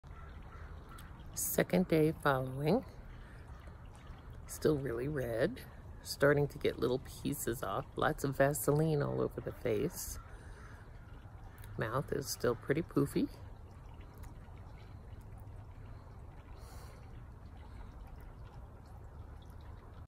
Day 2 after the Partial Chemical Peel September 2019
2nd day after a partial chemical peel.
September 2019